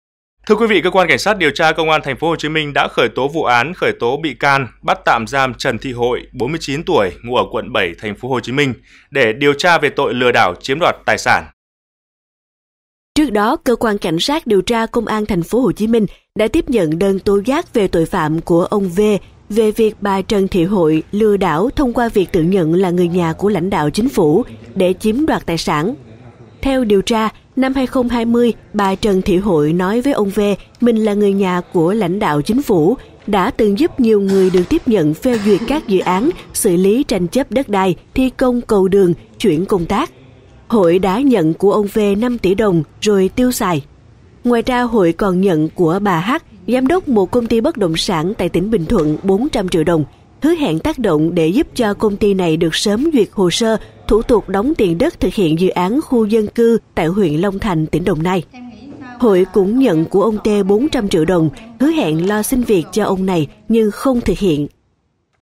Nhận là người nhà lãnh đạo Chính phủ để lừa chạy dự án, chiếm đoạt 6 tỉ đồng
Bà Trần Thị Hội tự nhận là người nhà của lãnh đạo Chính phủ, có khả năng 'chạy dự án', 'chạy xin việc' để lừa đảo chiếm đoạt 5,8 tỉ đồng.